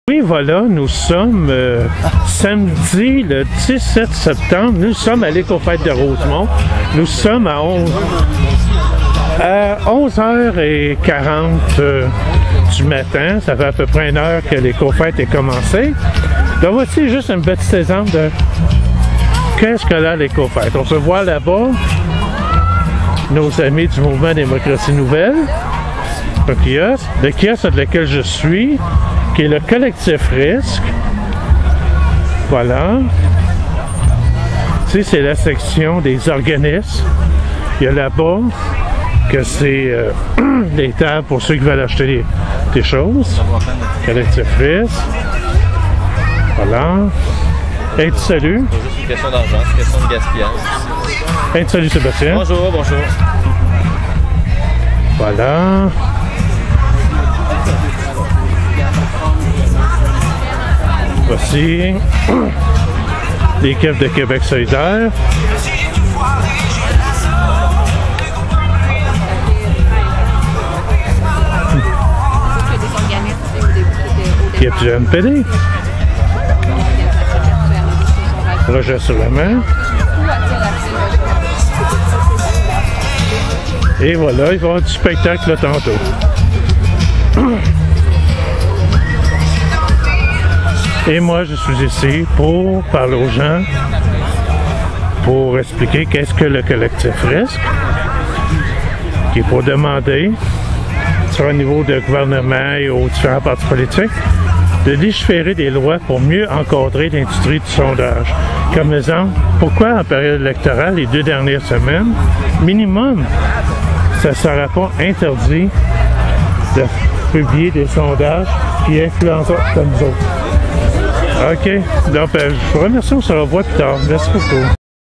L'Écofête de Rosemont 2016
Ce samedi 17 septembre c'était l'édition 2016 de l'Écofête.

Voici un petit vidéo souvenir de l'Écofête :-)